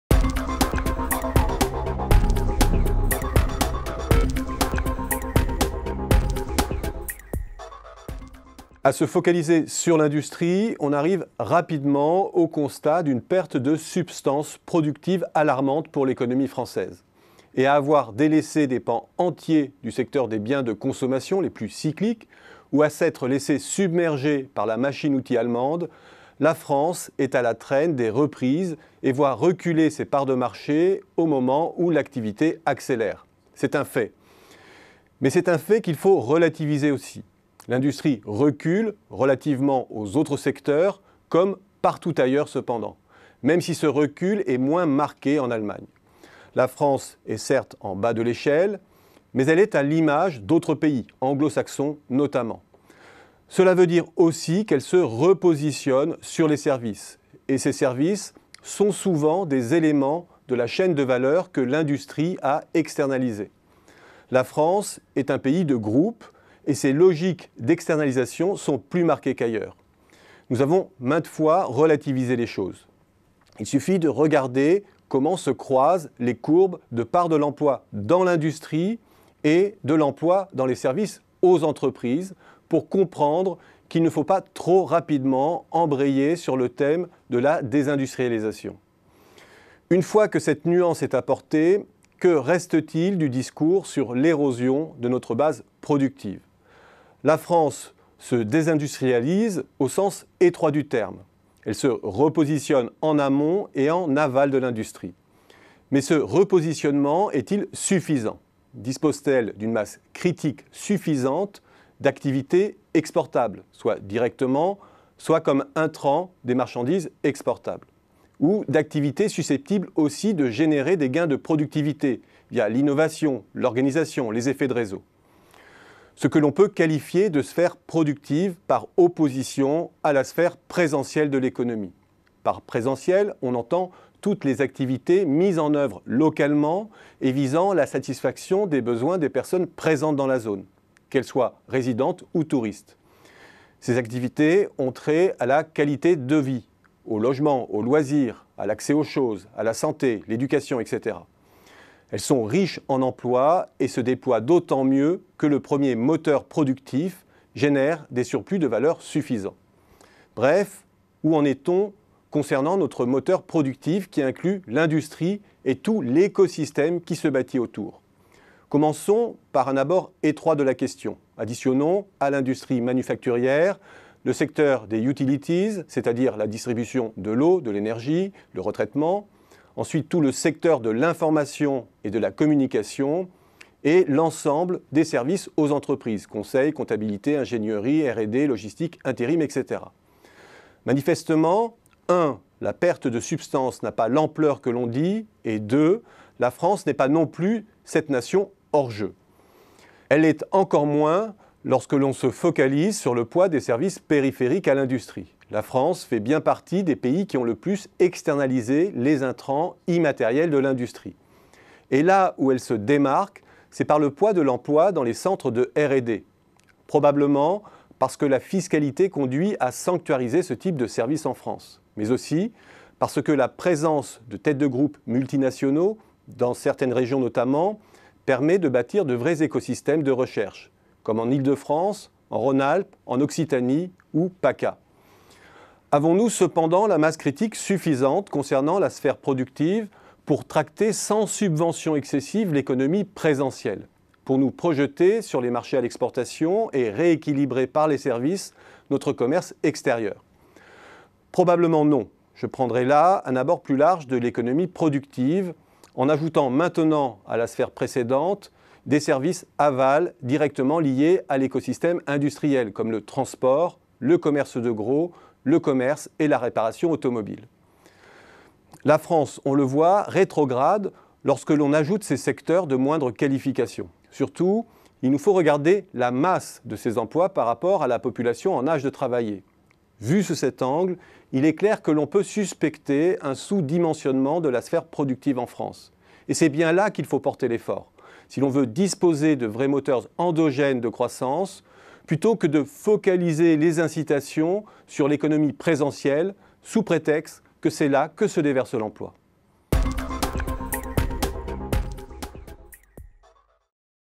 Il faut accélérer le développement des services productifs [Olivier Passet]
A se focaliser sur l’industrie, on arrive rapidement au constat d’une perte de substance productive alarmante pour l’économie française. En délaissant des pans entiers du secteur des biens de consommation les plus cycliques, en se laissant submerger par la machine-outil allemande, la France est à la traine de la reprise et voit reculer ses parts de marché au moment où l’activité accélère. C’est un fait.
Mais c’est un fait à relativiser. [...]